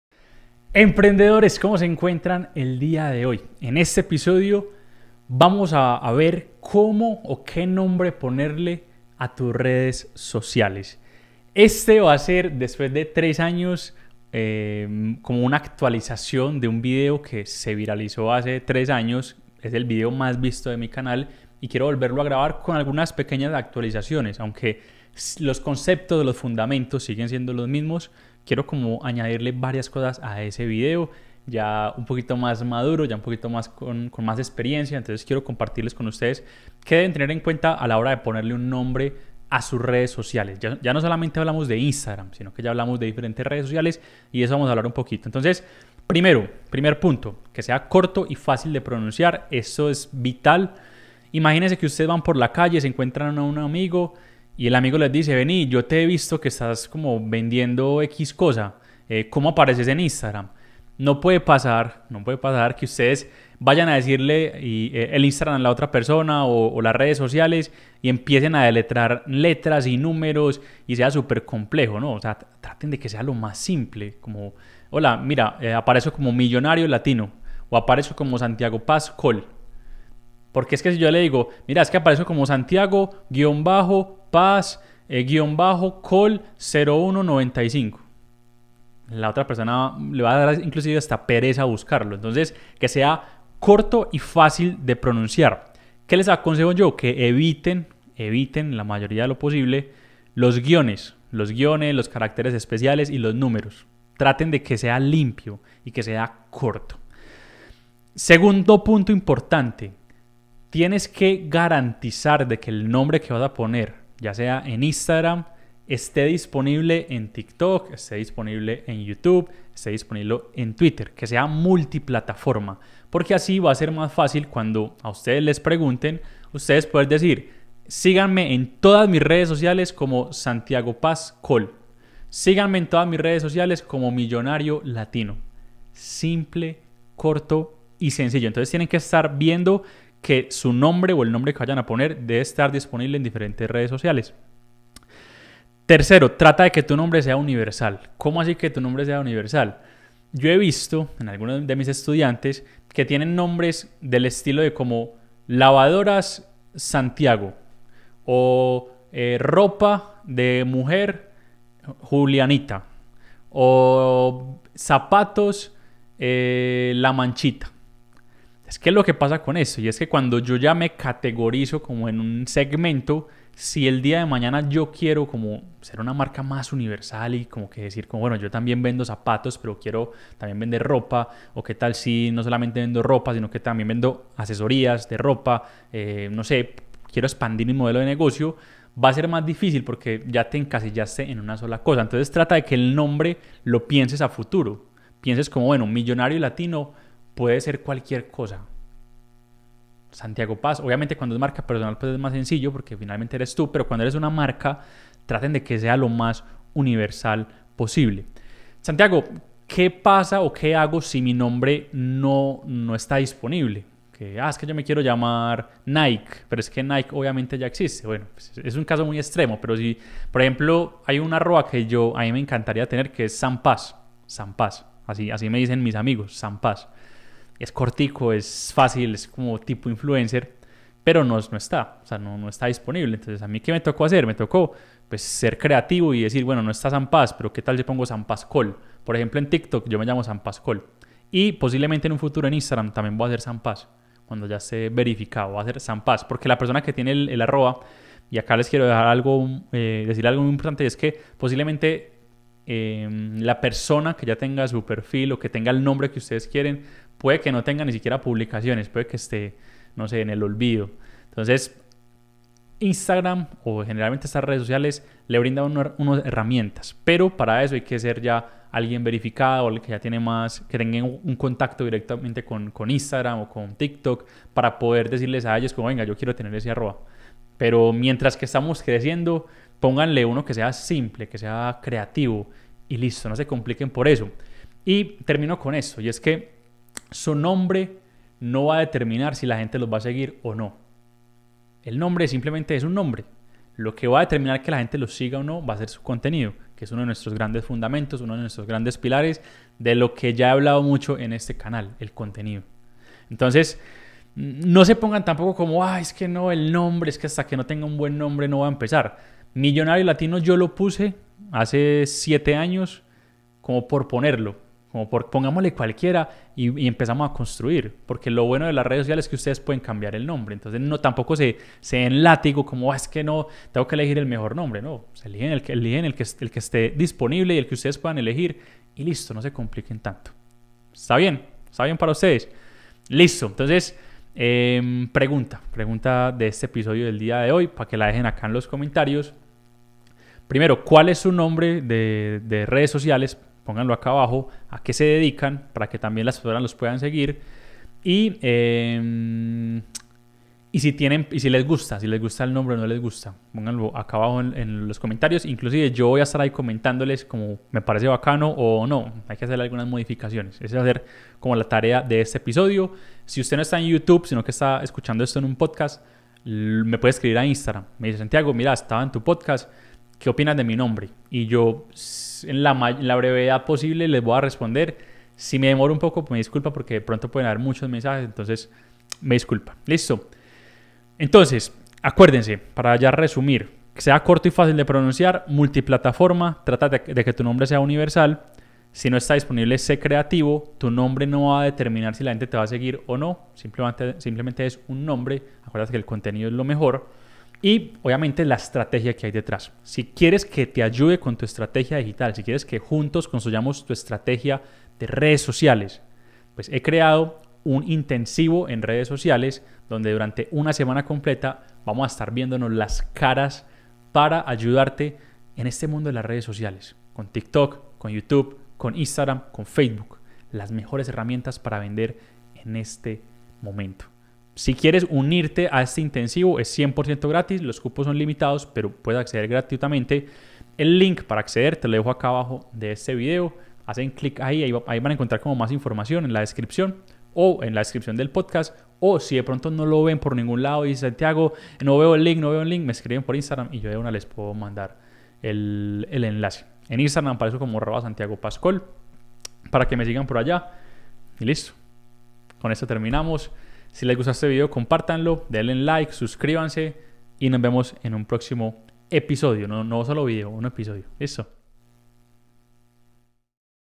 ¿Cómo Elegir un BUEN Nombre en Tu Cuenta de Redes Sociales? [El Mejor Nombre para tu INSTAGRAM]
En este video aprenderás Cómo elegir un Buen Nombre en CUALQUIER Red Social